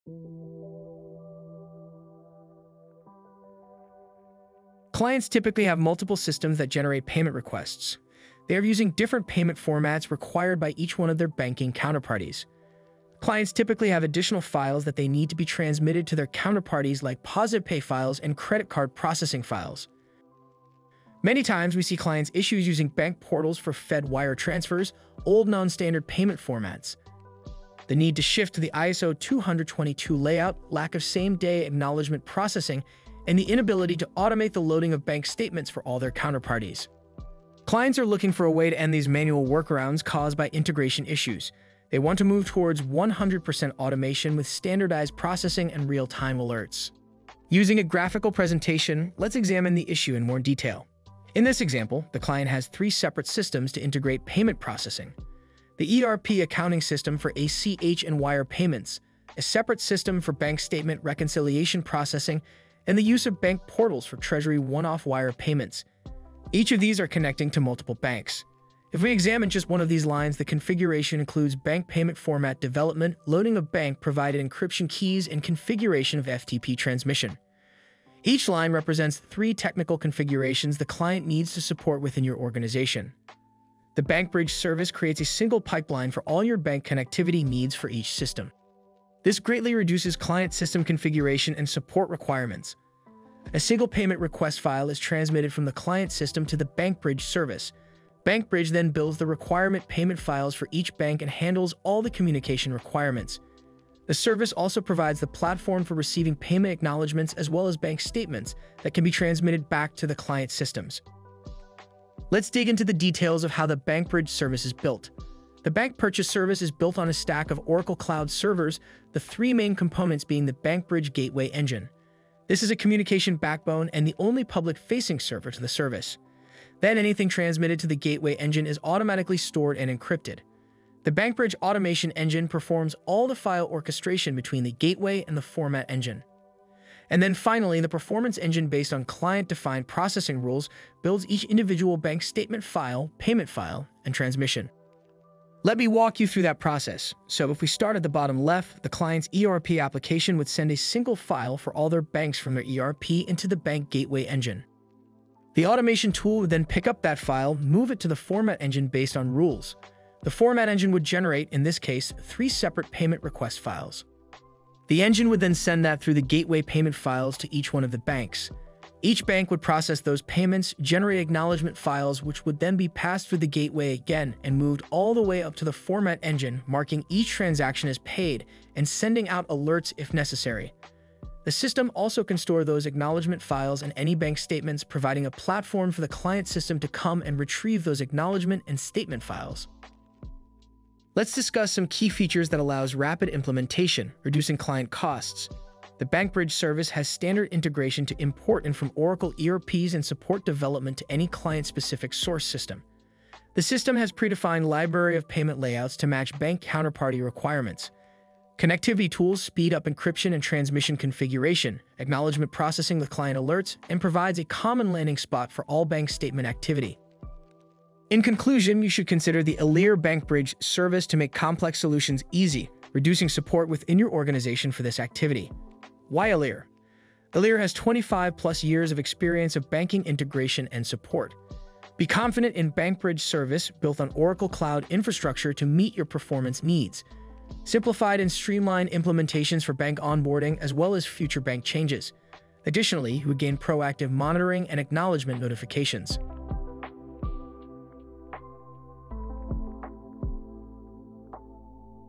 Elire BankBridge™ - Complex Solutions, Made Easy
The Elire BankBridge™ is a cloud-based connectivity hub providing a secure, single pipeline for straight-through processing of bank and other financial counterparty information. BankBridge™ supports real-time internet-based communication with banks and other financial institutions and includes industry-standard security protocols. BankBridge minimizes the burden on your IT Team, removing the need to develop and maintain bank connections and format updates in-house, using Elire's delivered payment library. BankBridge™ also enhances visibility for your organization with the ability to track transactions from End-to-End using source reference data for acknowledgment processing.

Simple. Streamlined. Secure.
BankBridge™ is one external connection for all your banking relationships.